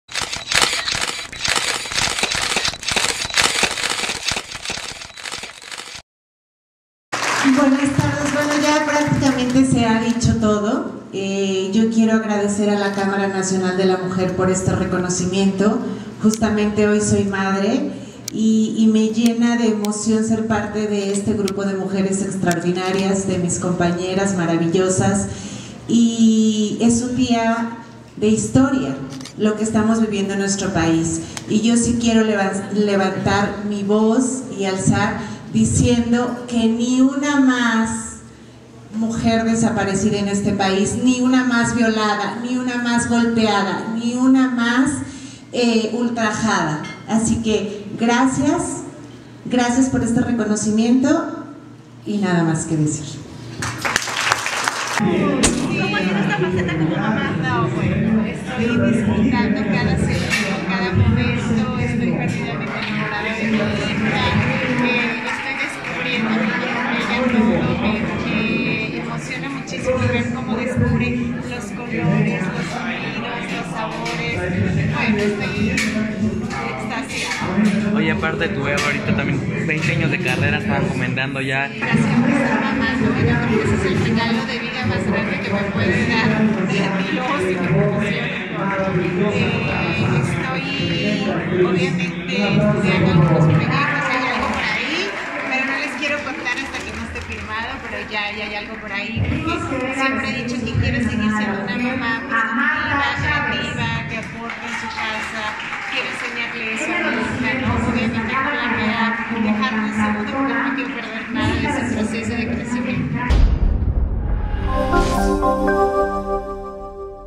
Marlene Favela recibió el Premio Nacional de la Mujer 2020
“Es un día histórico lo que estamos viviendo en nuestro país y yo si quiero levantar mi voz, diciendo que ni una mujer más  desaparecida en este país, ni una más violada, ni una más golpeada...”